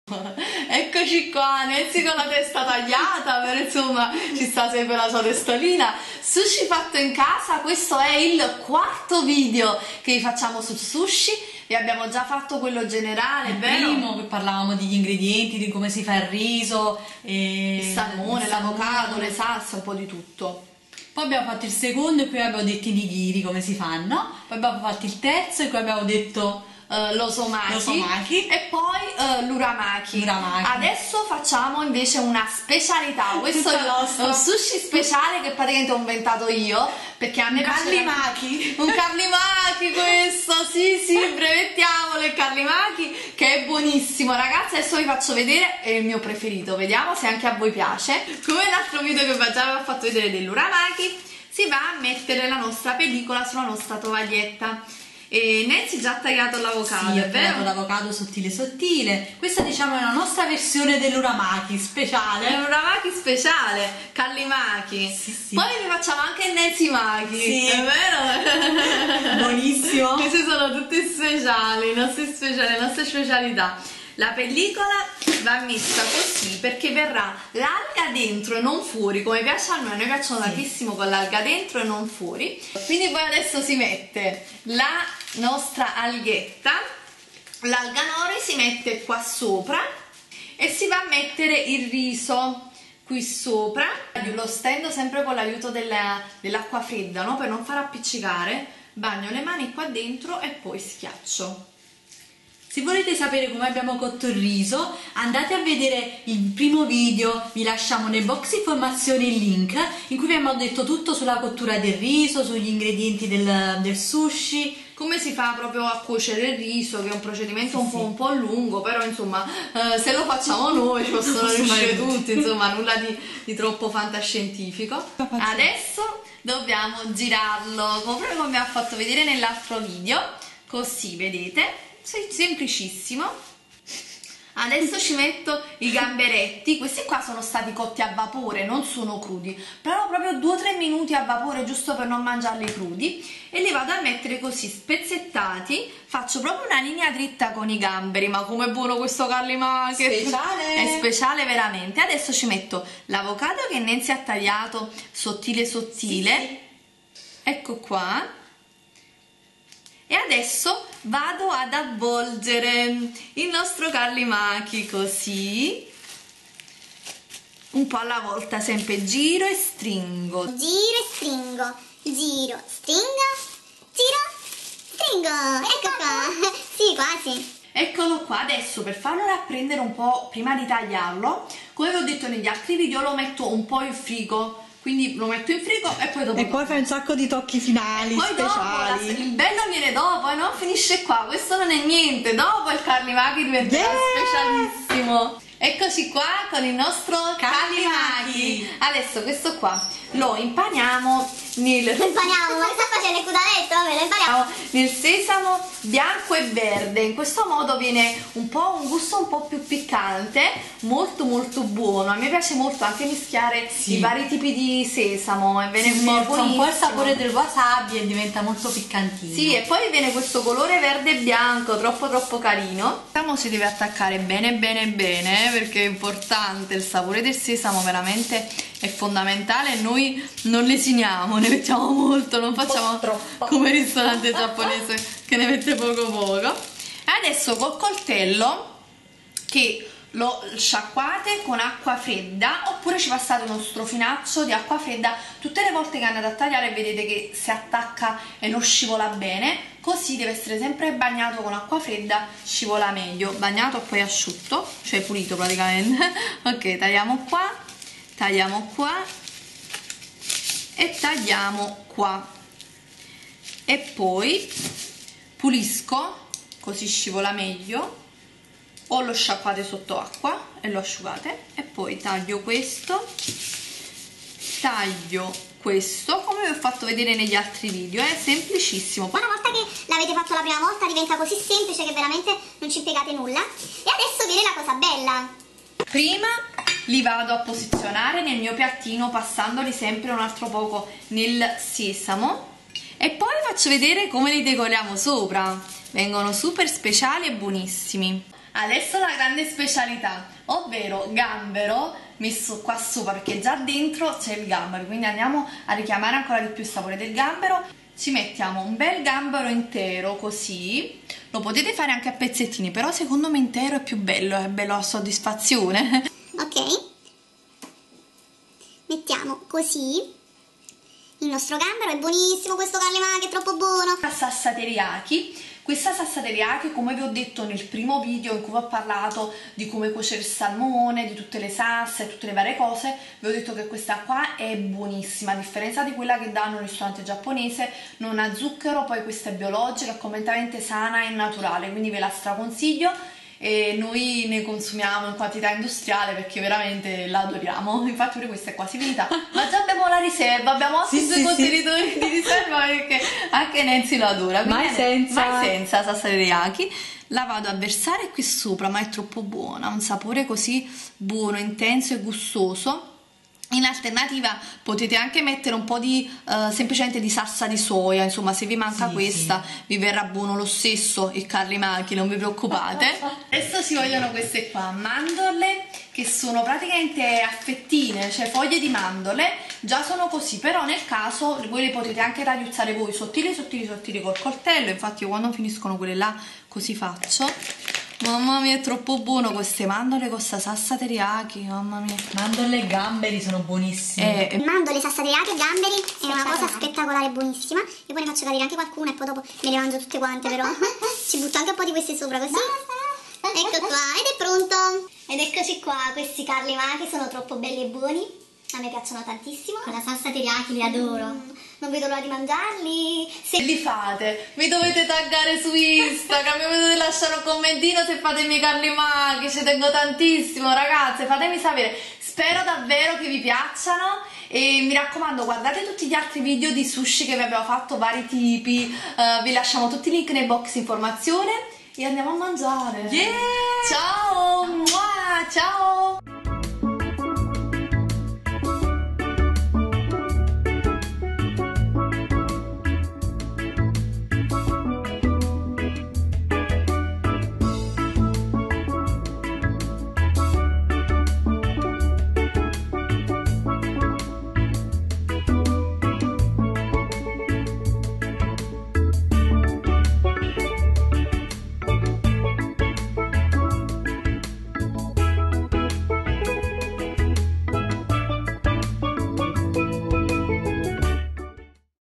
SUSHI Fatto in Casa | Sushi at Home #4 - CARLIMAKI Special Roll by CARLITADOLCE
Ciao a tutti! Ecco una nuova video ricetta Healthy, sana e saporita, per mangiare bene, con gusto e restare in forma e in salute. Spero vi piaccia... Buon Appetito! :-)